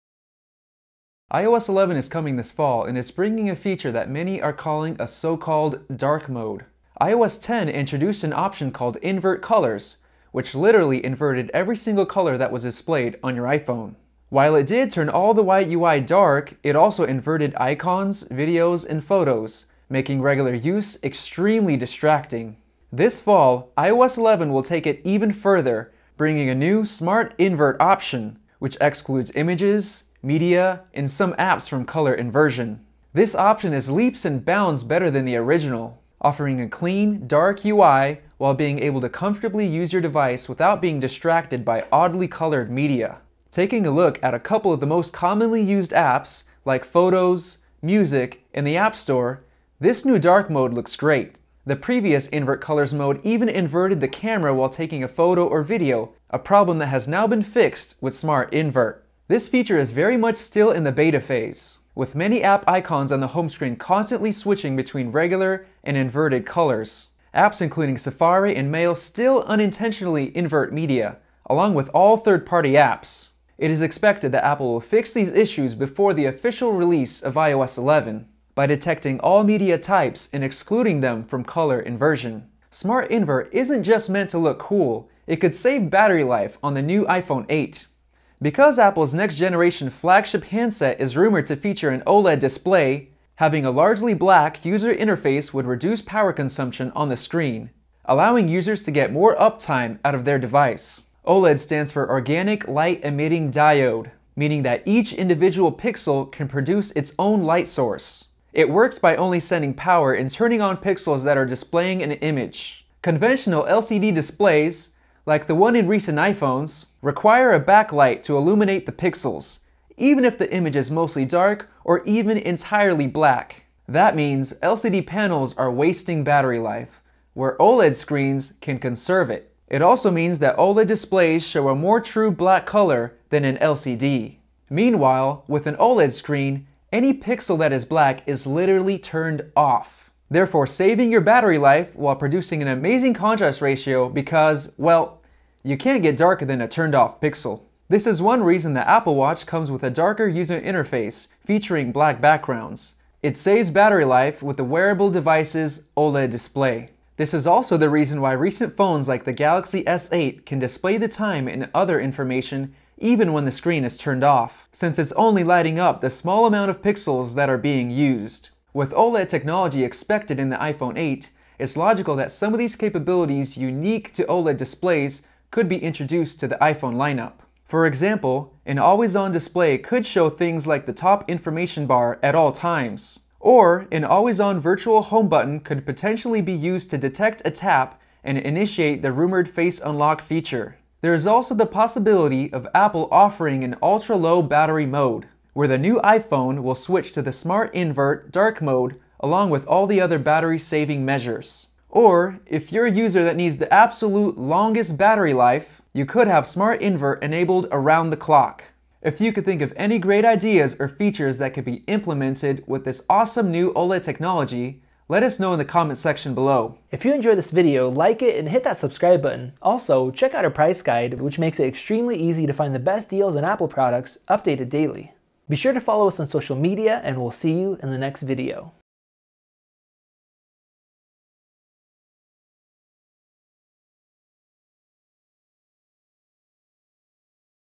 How to turn on 'Dark Mode' on iPhone!
When iOS 11 launches this fall, it will include Apple's version of 'Dark Mode,' dubbed Smart Invert. The feature inverts screen colors except for media, such as photos, videos, and icons, a handy option that might be used to extend battery life on the upcoming OLED-equipped 'iPhone 8.'